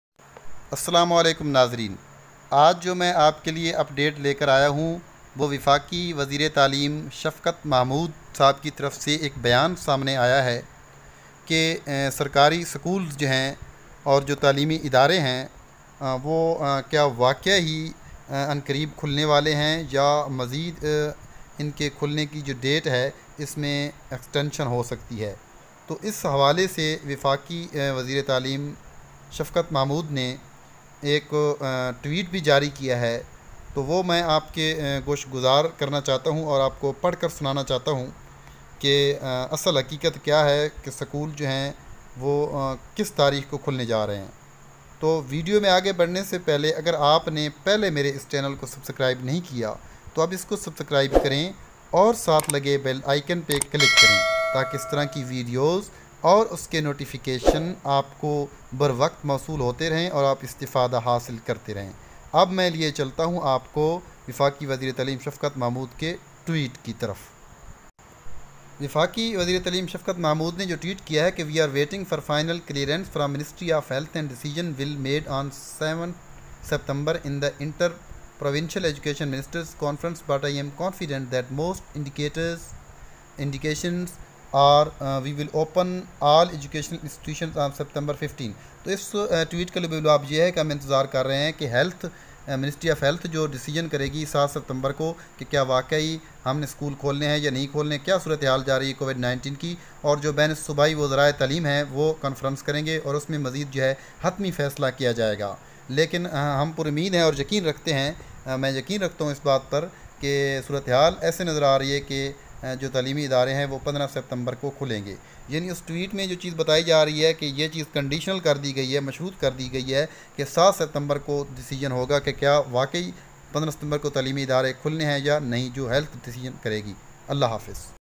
When Schools Will Open?||Federal Education Minister Tweet||World of Knowledge INQ.
This vidio is about Federal Education Minister Tweet for schools opening.